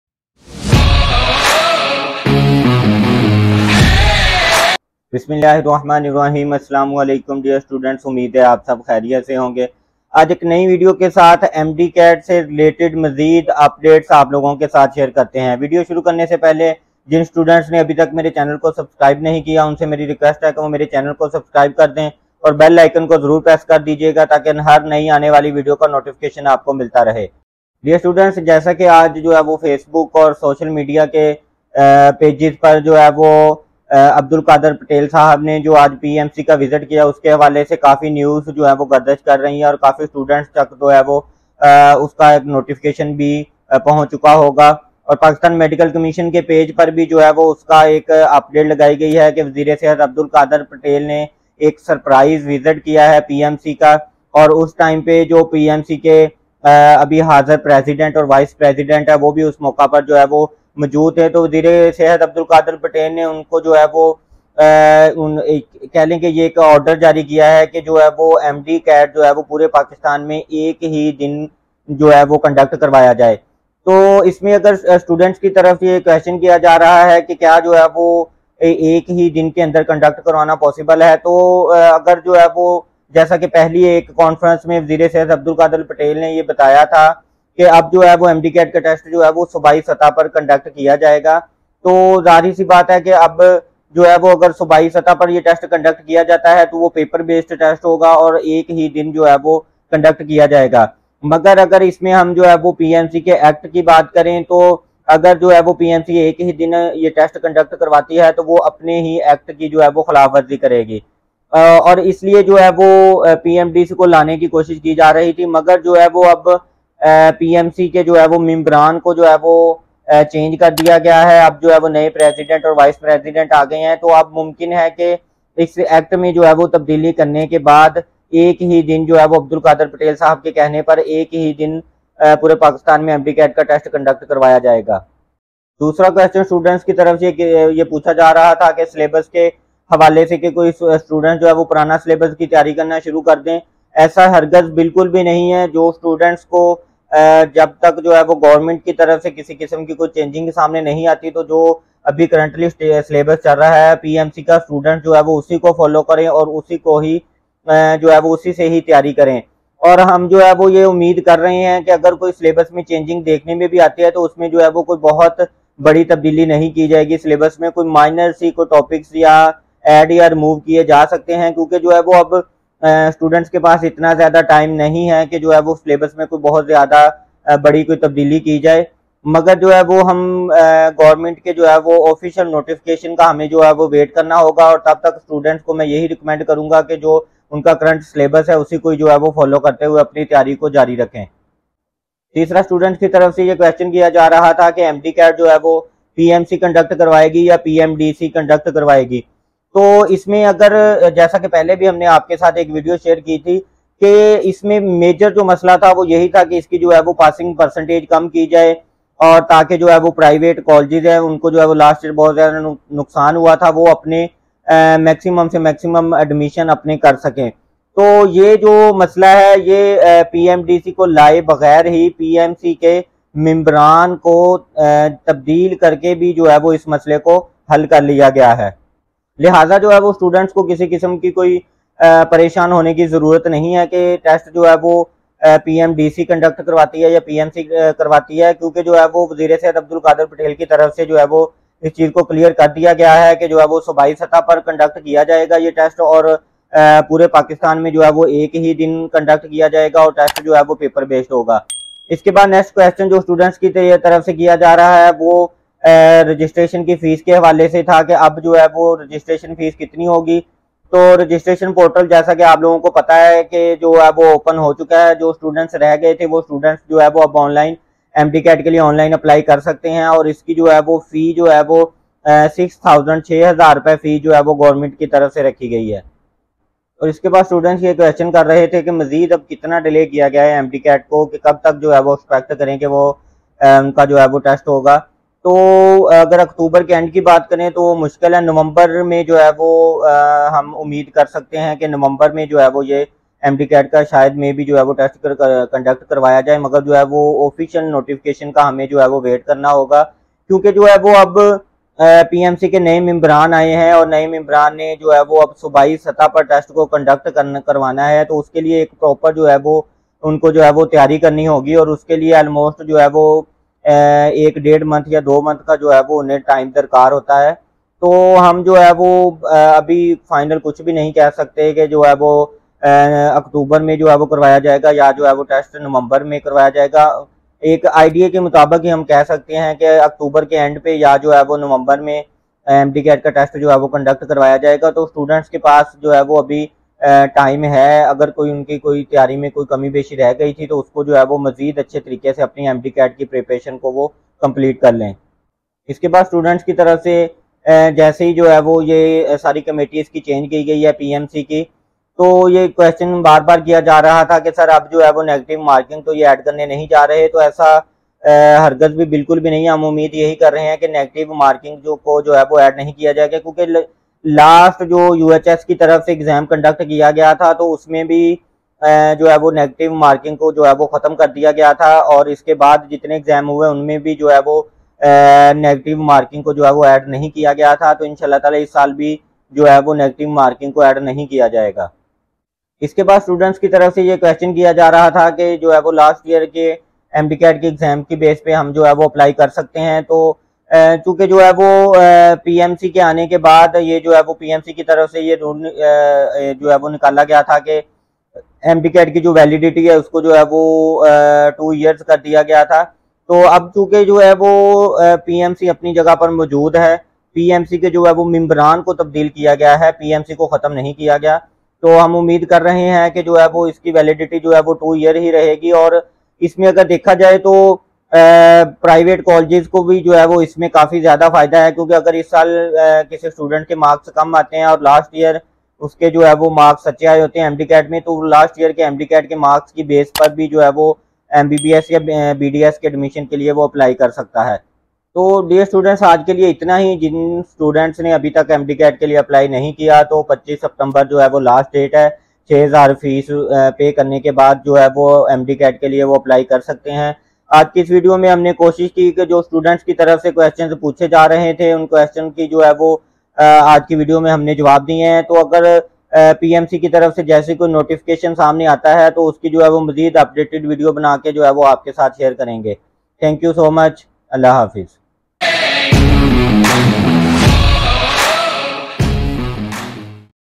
MDCAT on single Day in pakistan | All MDCAT updates | Pmdc
Federal Minister for Health Abdul Qadir Patel
paid a surprise visit to Pakistan Medical
Commission (PMC) and expressed satisfaction
on the working. The Federal Minister directed
PMC to hold MDCAT Exam on single day
throughout Pakistan. The government is working
to conduct MDCAT Examination on provincial
level, paper based and on single day.
Many students have started preparing from the
previous provincial syllabus of 2019 in
anticipation that the syllabus would be changed.
But the students should stick to the PMC
syllabus unless and official notification is issued
regarding the change in the syllabus. This video
provides complete information about all the
MDCAT updates so far. The following questions
are addressed in the video:
~ Will MDCAT Exam be conducted on National
or Provincial level?
~ Which Authority would conduct MDCAT in
2022? PMC or PMDC?
~ What is the expected schedule of MDCAT
2022?
~ Which syllabus students should follow? Is
there any chance of change in the syllabus?
~ Will negative marking be included in Provincial
MDCAT?
~ Can the students get admission on the basis
of last year MDCAT? Is MDCAT validity still 2
years?
~ What is the last date to apply for MDCAT 2022